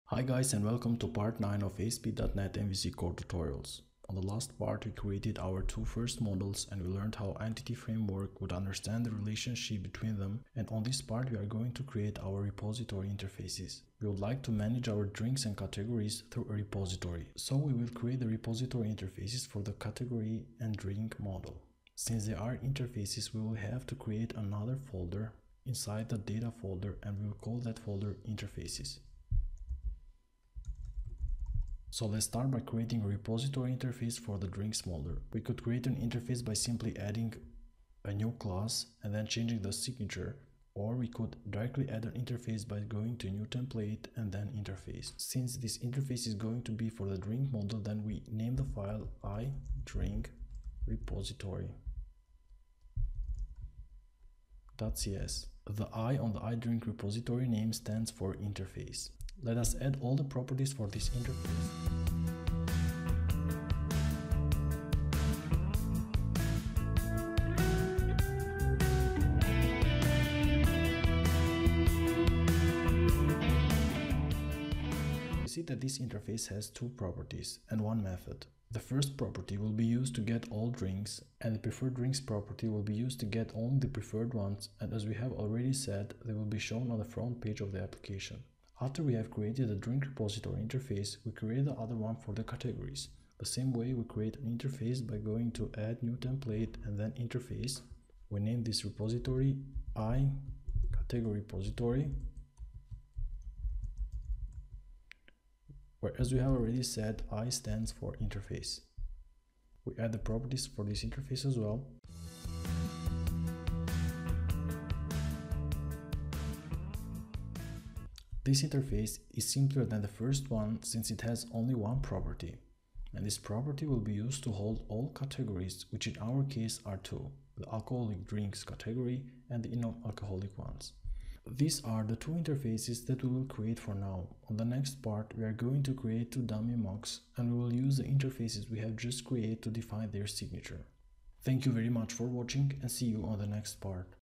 9. REPOSITORY INTERFACES - Asp.Net CORE MVC
On this part you will learn how to add the repository interfaces to the project.